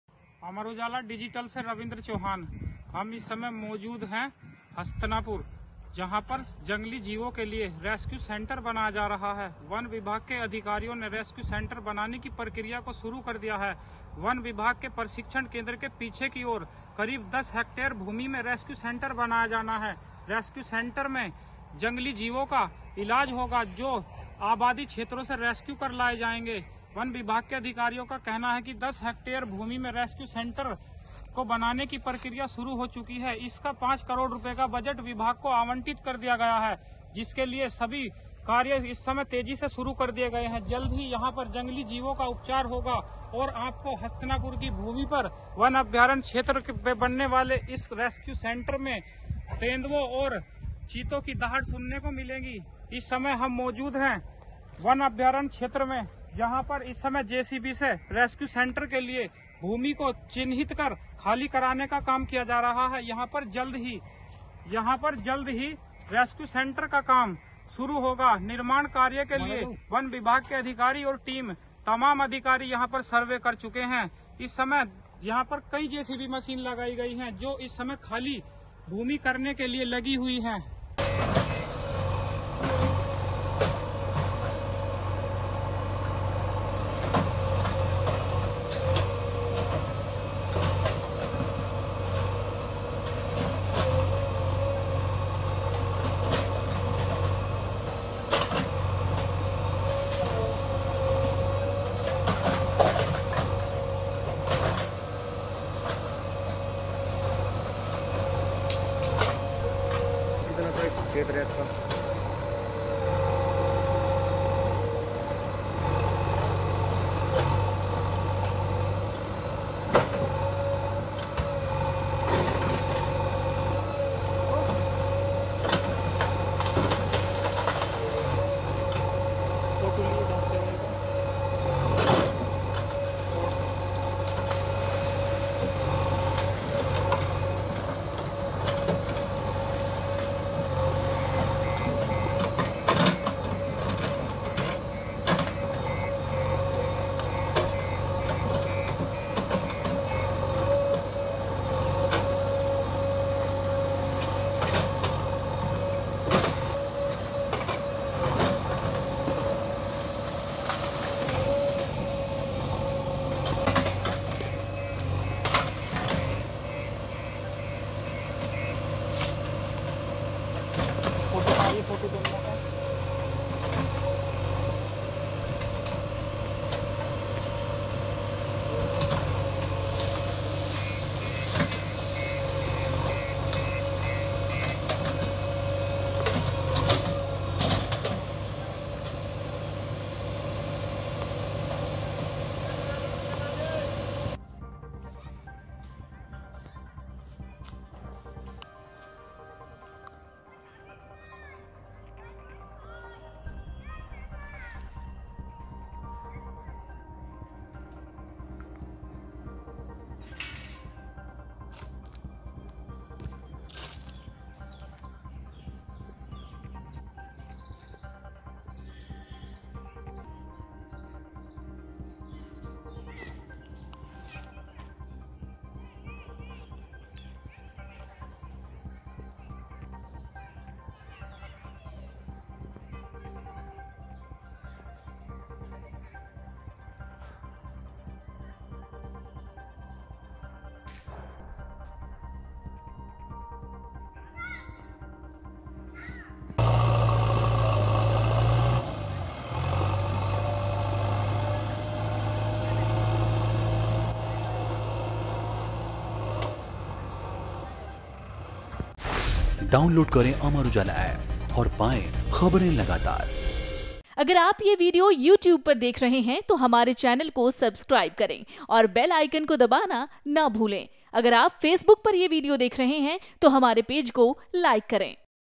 जंगली जीवो के लिए हस्तिनापुर में शुरू हुआ रेस्क्यू सेंटर का काम, शासन से मिली अनुमति
मेरठ में वन विभाग की हस्तिनापुर रेंज में वन्यजीव रेस्क्यू सेंटर बनाए जाने की शासन से अनुमति मिलते ही टेंडर प्रक्रिया भी पूर्ण हो गई है। सभी प्रक्रिया पूर्ण होने के बाद रेस्क्यू सेंटर का निर्माण कार्य शुरू किया गया है जिससे मेरठ बिजनौर सहारनपुर मुजफ्फरनगर अमरोहा आदि जिलों में रेस्क्यू किए गए जानवरों को हस्तिनापुर में बनने वाले रेस्क्यू सेंटर में लाया जाएगा।